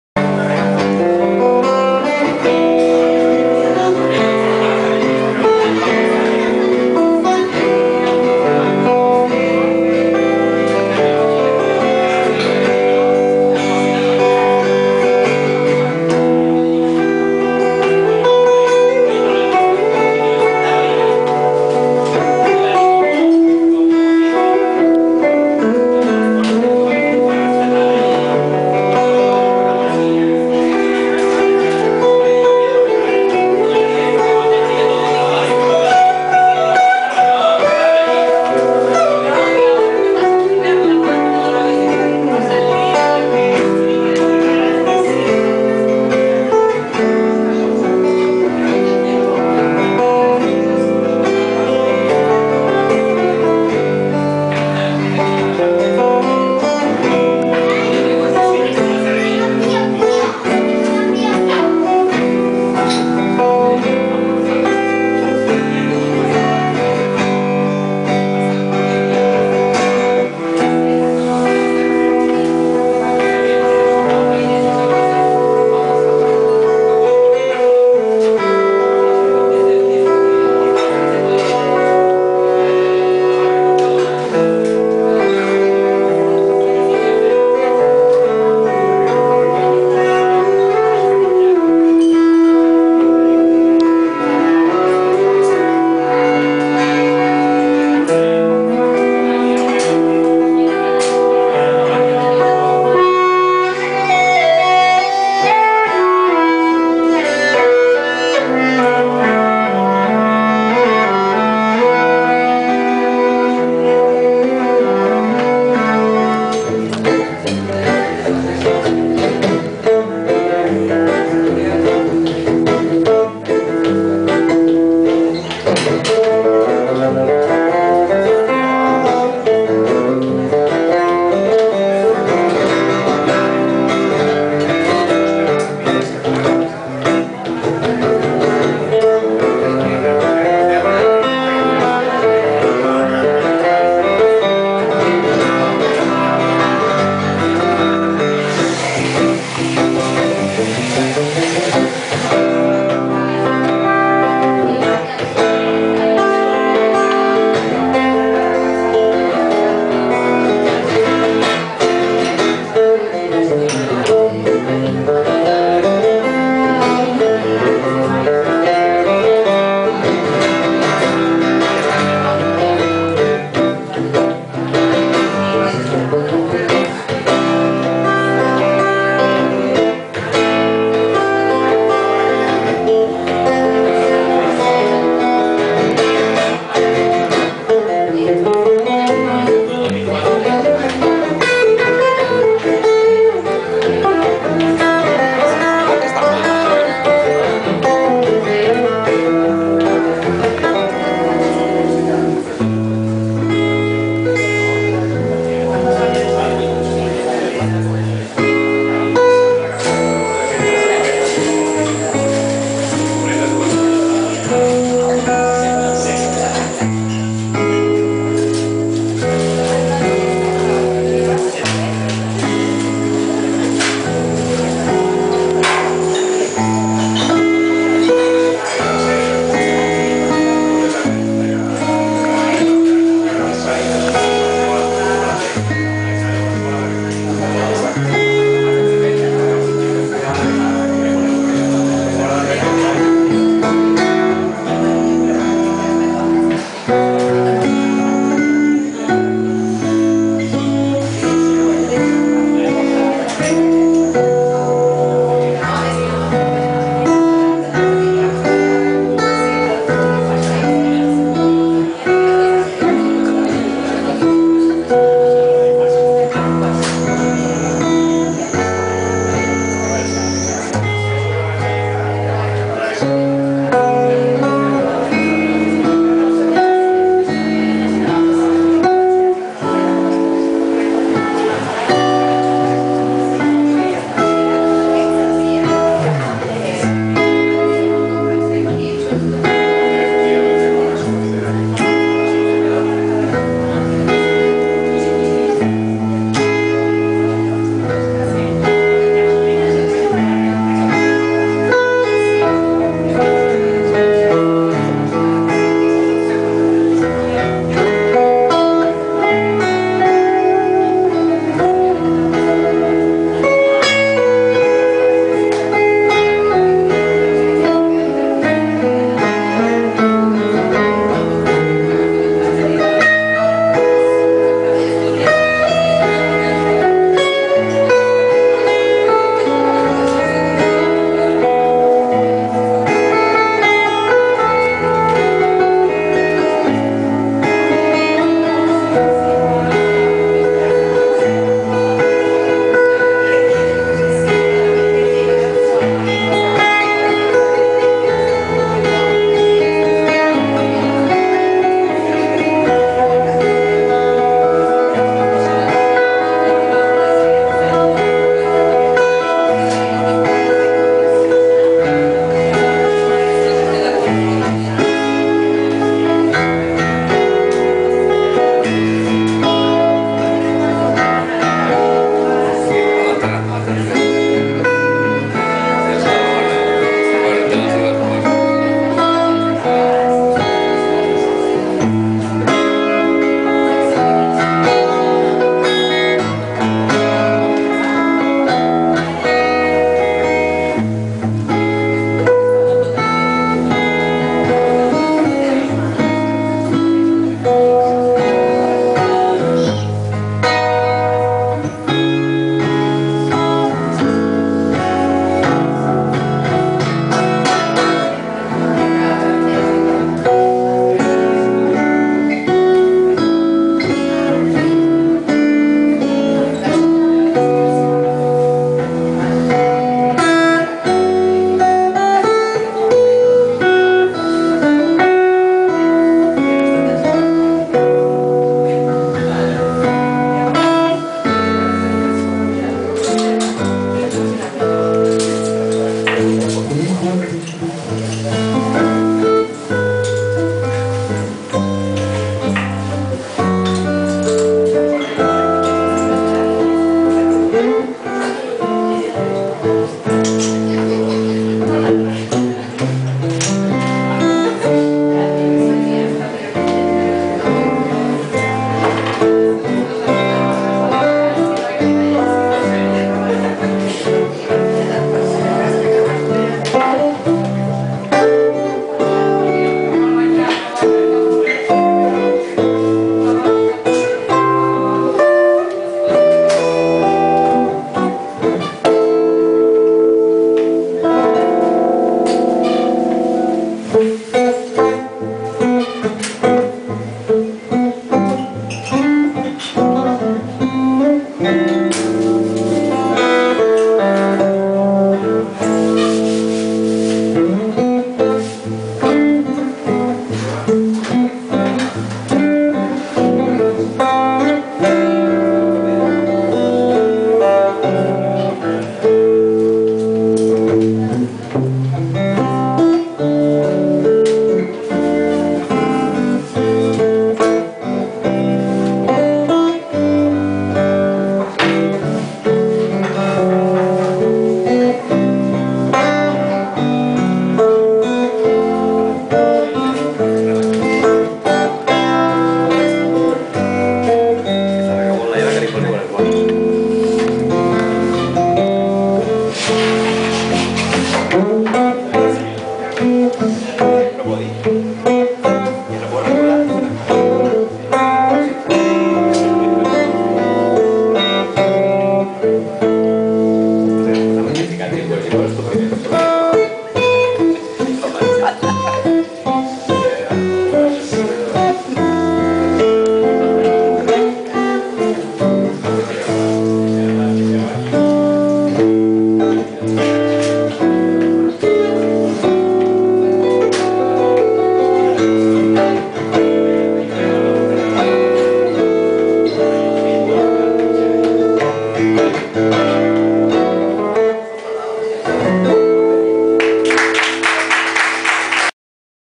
PEP ROMAGUERA A SANT CEBRIÀ DE VALLALTA
UN CONCERT EN EL BAR DEEP
La interpretació, nua, sense intercanvis a lescenari, sense paraules, provoca lespectador, que disfruti sentint -no sols escoltant- i també veient com la música es construeix artesanal-ment amb un virtuosisme modest.